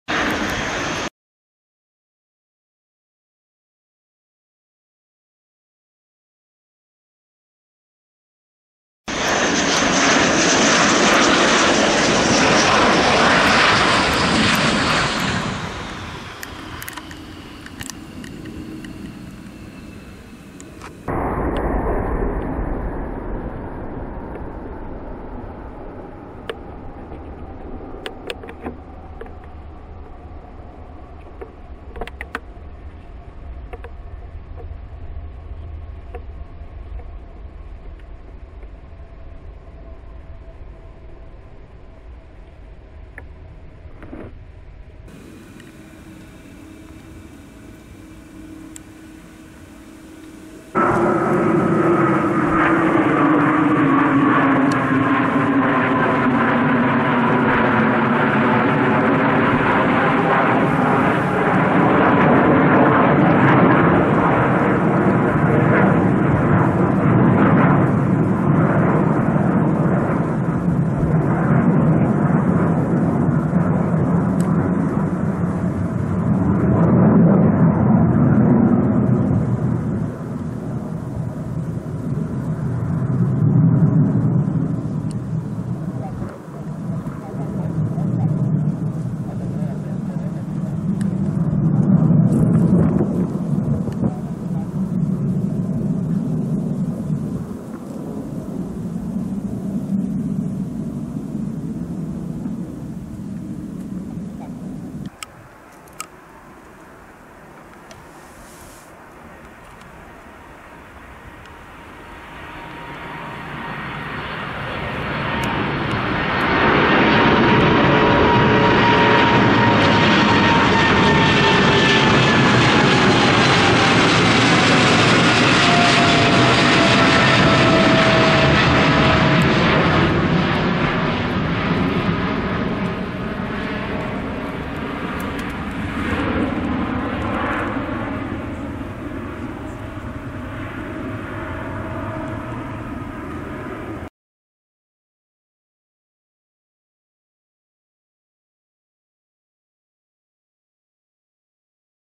千歳基地 天狗のフックランディング
基地で雑談中に南風運用なのに南からエンジン音が聞こえてきました。
え？っと思って南を見るとイーグルが降りて来ます。
慌てて撮影したらフック出していてしかも天狗さんでした。
その後、４機編隊などもあり満足した金曜日となりました。